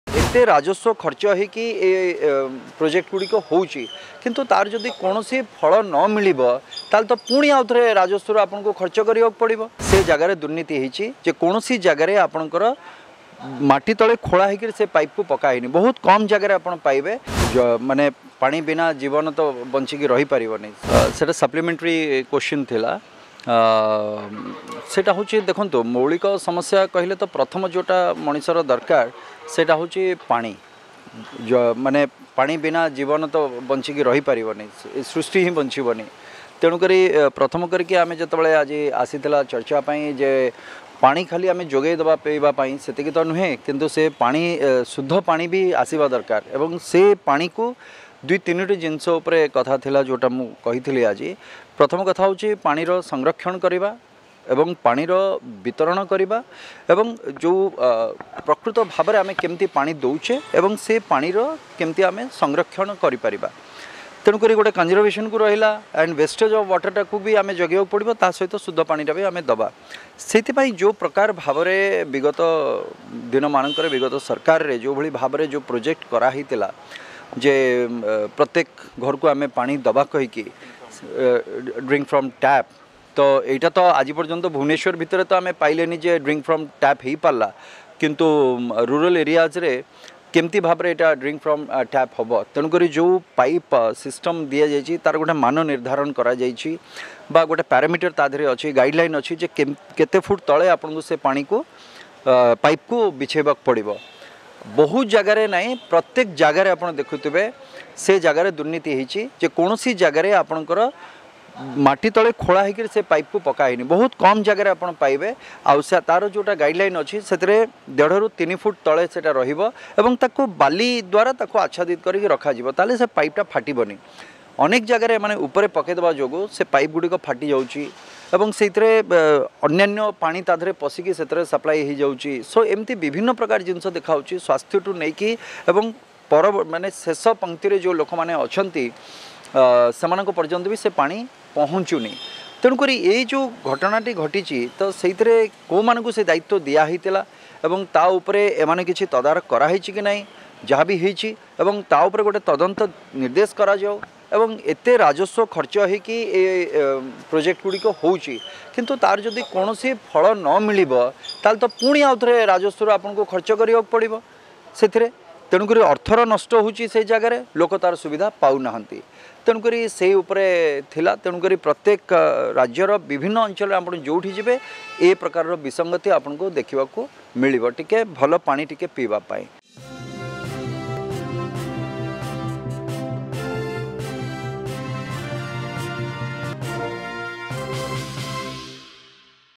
ଡ୍ରିଙ୍କ୍ ଫ୍ରମ୍ ଟ୍ୟାପ୍...ରାଜସ୍ୱ ଖର୍ଚ୍ଚ ହେଇକି କାମ ହେଇଛି ହେଲେ କିଛି ଫଳ ମିଳିନି: ସିଦ୍ଧାନ୍ତ ମହାପାତ୍ର
ଡ୍ରିଙ୍କ୍ ଫ୍ରମ୍ ଟ୍ୟାପ୍...ରାଜସ୍ୱ ଖର୍ଚ୍ଚ ହେଇକି କାମ ହେଇଛି ହେଲେ କିଛି ଫଳ ମିଳିନି:  ସିଦ୍ଧାନ୍ତ ମହାପାତ୍ର, ବିଜେପି ବିଧାୟକ